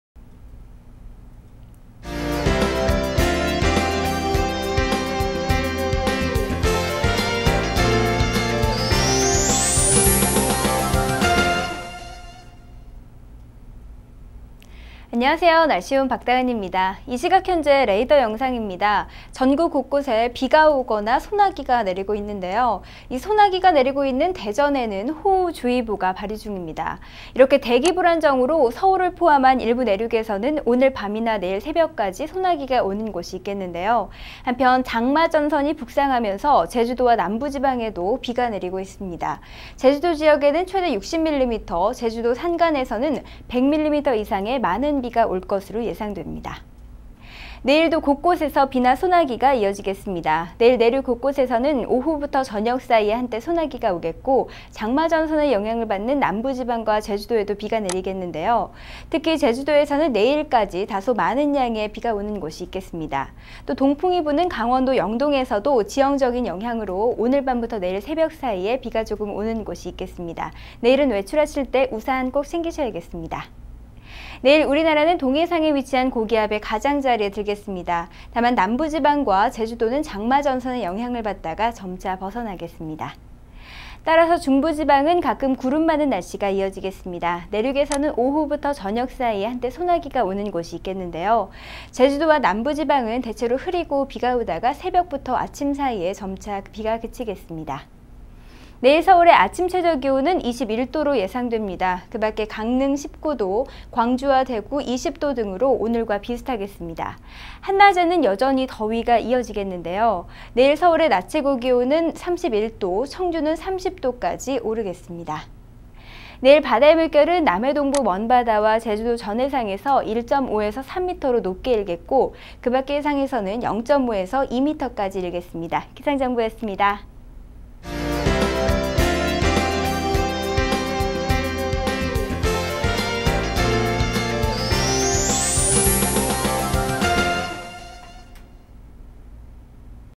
날씨정보 6월 25일 17시 발표
▶ 내일(금)
-  중부지방 가끔 구름 많음
    내륙지역 오후~저녁: 한때 소나기
    제주도, 남부지방: 대체로 흐리고 비 ... 새벽~아침 점차 그침
    강원도 영동: 새벽까지 비 조금